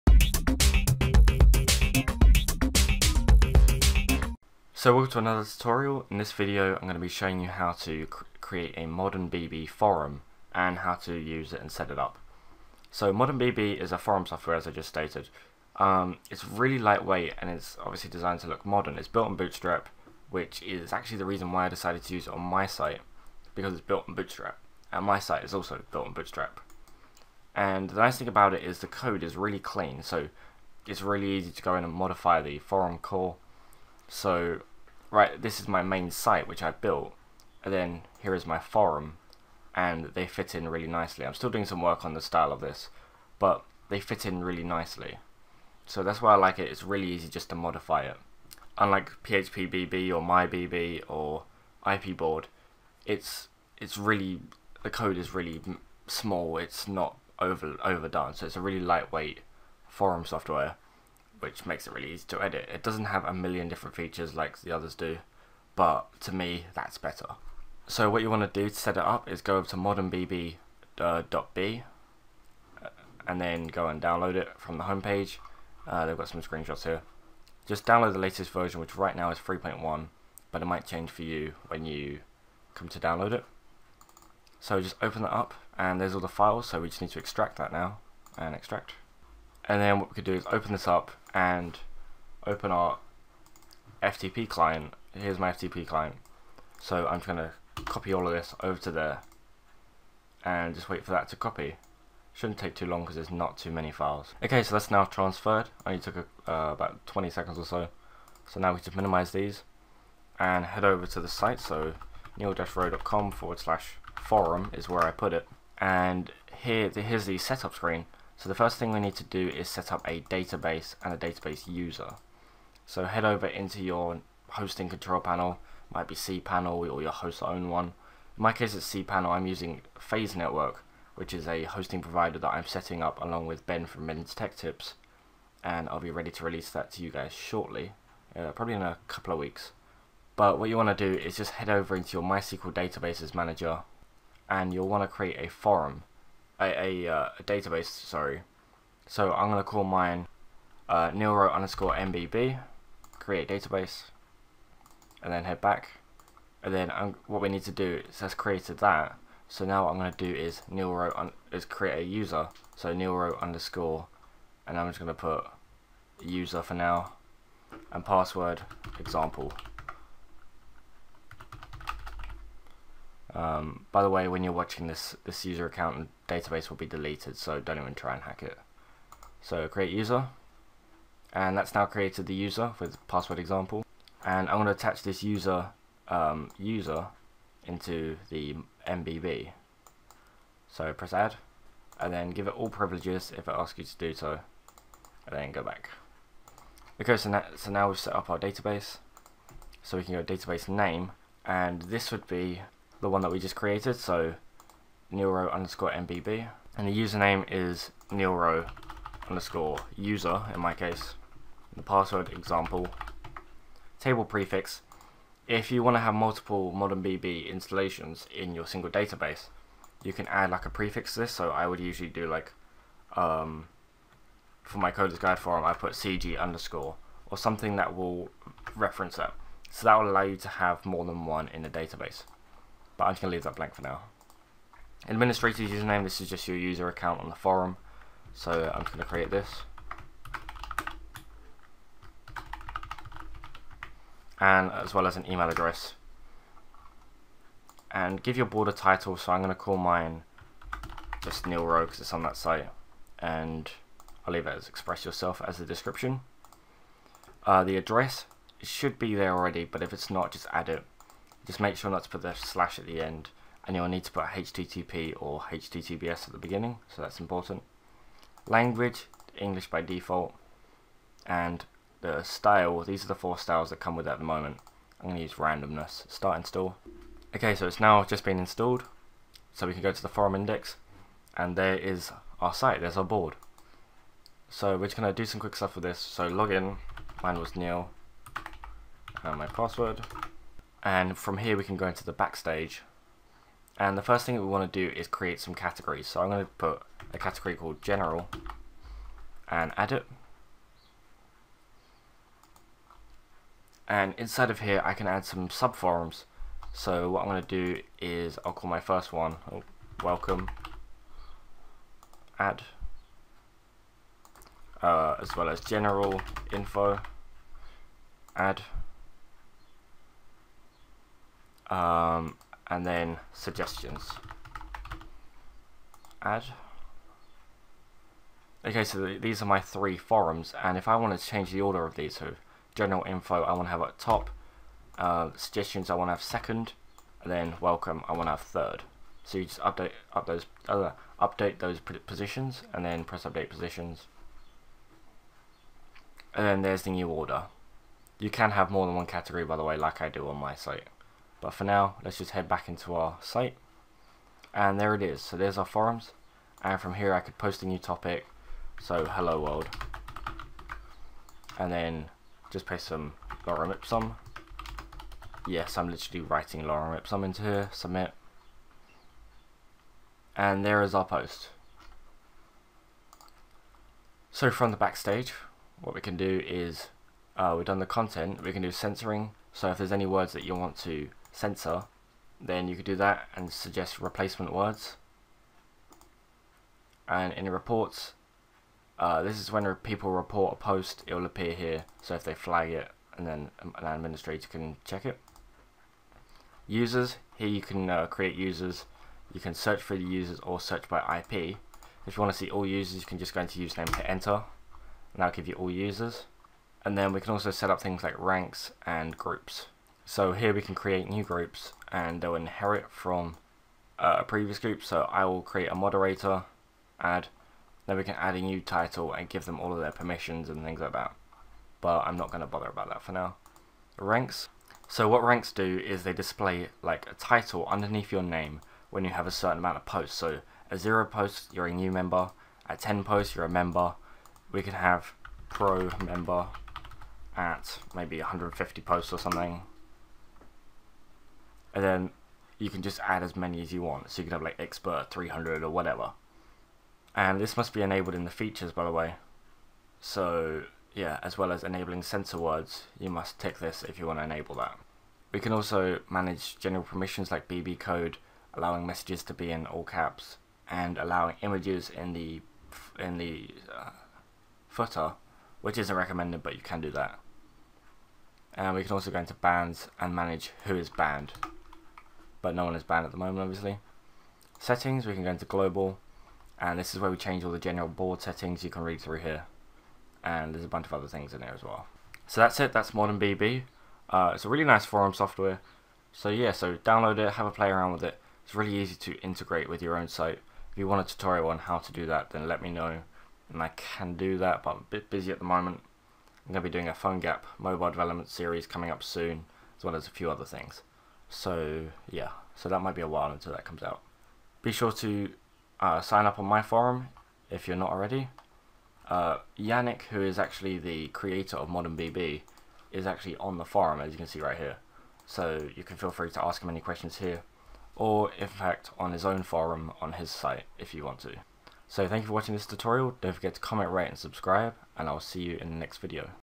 ModernBB Install / Setup Tutorial (Bootstrap Forum)
ModernBB is a lightweight but beautiful forum board which is built on Bootstrap 3. It is the system that powers the forum on coders-guide.com too!